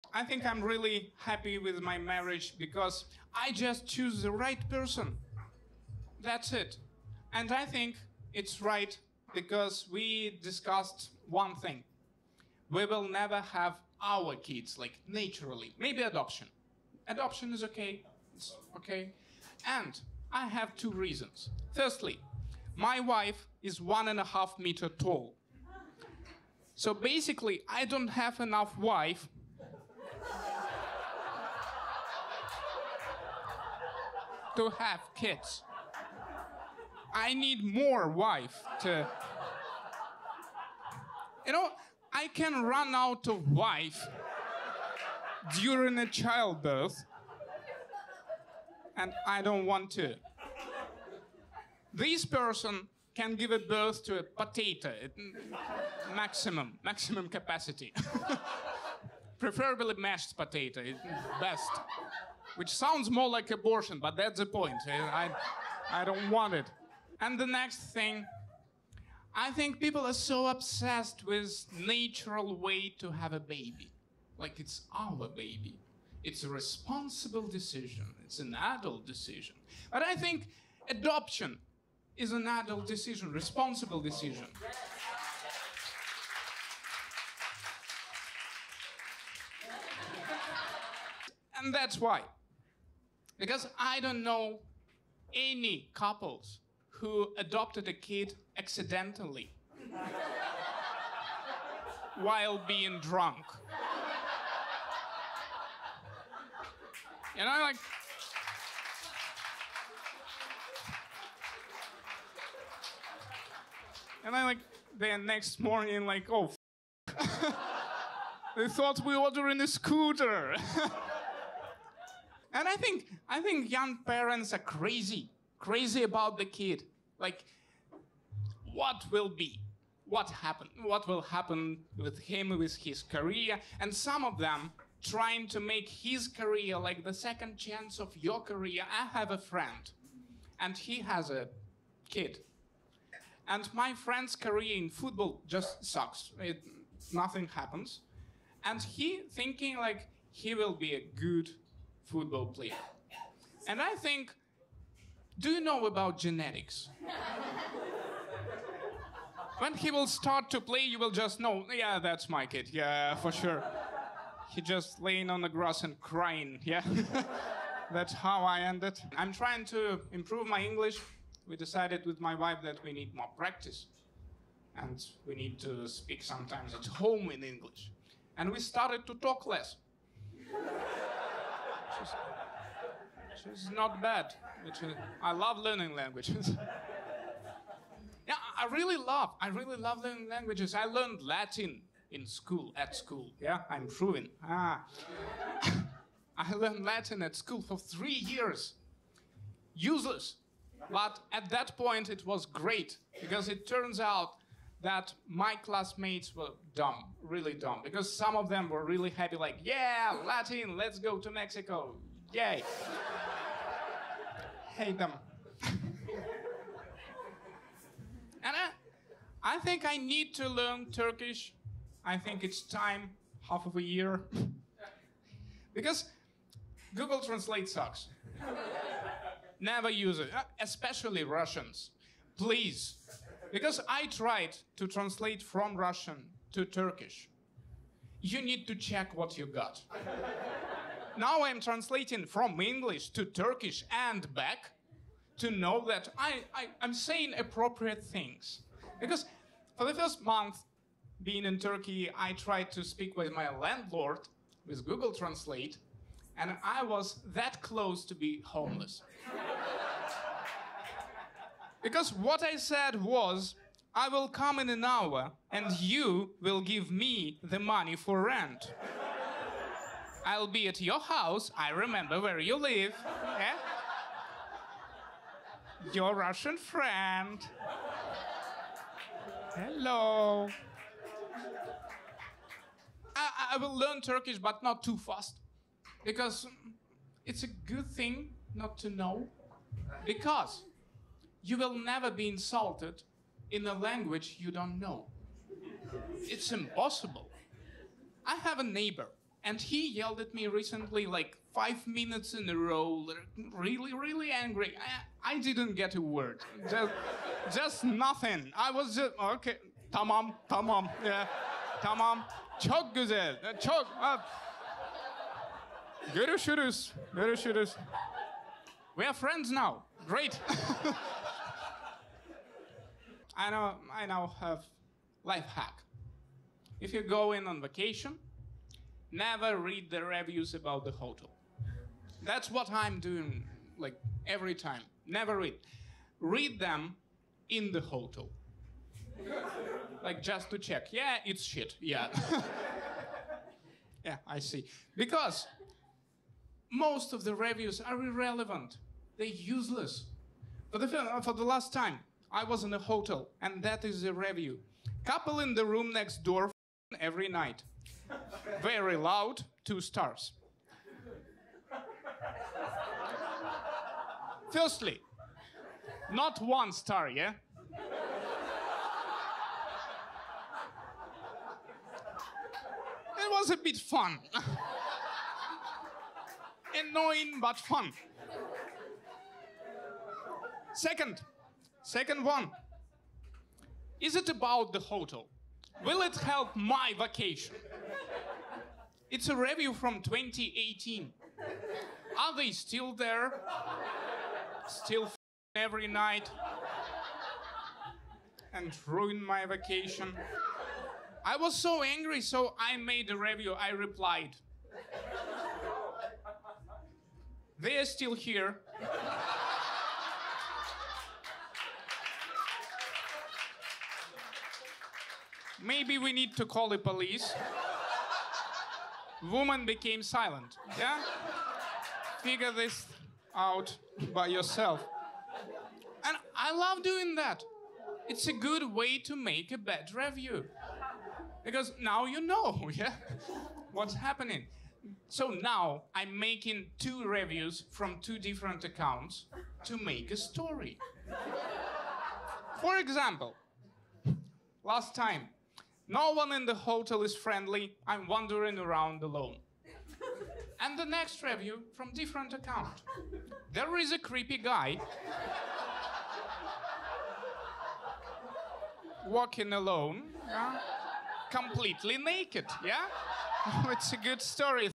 I don't want kids | Stand-Up 2023
Hi there, I'm Russian comedian in Turkey, and here i'm talking about my reasons to not have kids. If you want them - write in comments why. If you agree with me - tell me how right I am. Cheers.

And I perform in English for some time, but still not sure about my accent, so there are subtitles for you :)

00:00 - we will never have kids
00:26 - first reason
01:07 - second reason
02:02 - young parents are crazy
02:49 - I'm trying to improve my English
03:08 - I learned Latin
03:39 - I need to learn Turkish
04:43 - but not too fast
05:23 - lifehack for a good vacation